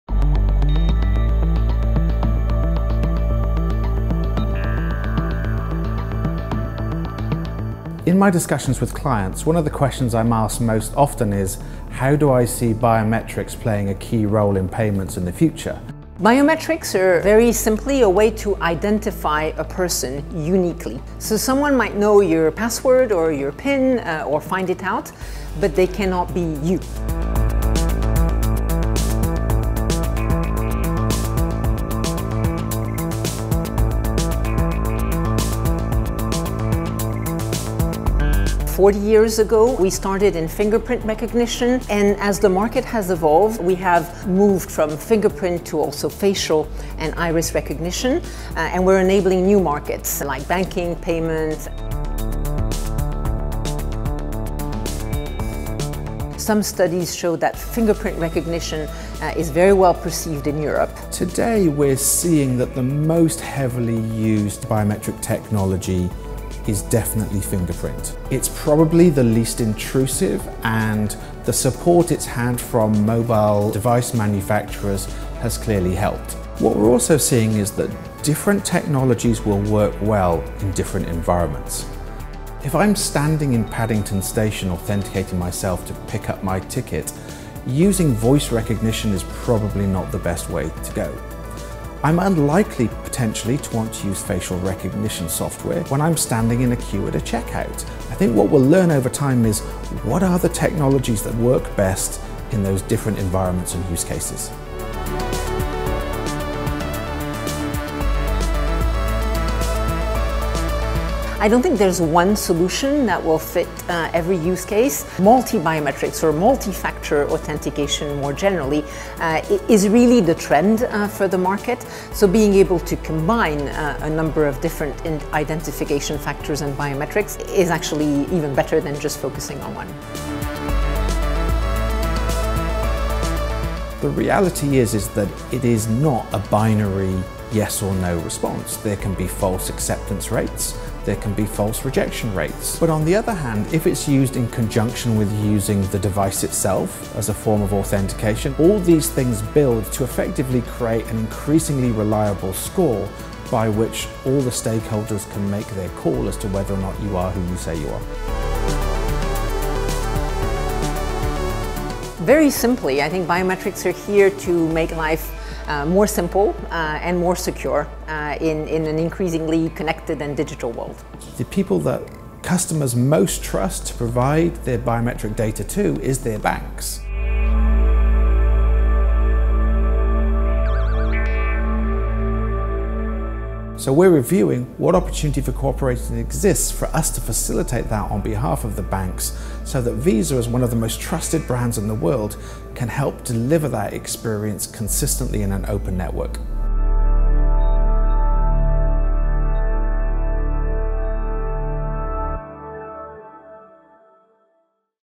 Visa Biometrics Study 2016
Biometric identification and verification has created a great deal of excitement in the payments space because it offers an opportunity to streamline and improve the customer experience. Visa research shows that biometrics is increasingly recognised as a trusted form of authentication as people become more familiar with using these capabilities on their devices.